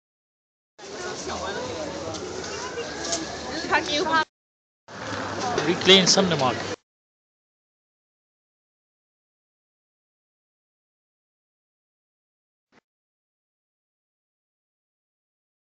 VERY BUSY BRICKLANE MARKET MUST GO TO SEE!!!
THIS MARKET IS EVERY SUNDAY, BRICKLANE, LONDON, UK.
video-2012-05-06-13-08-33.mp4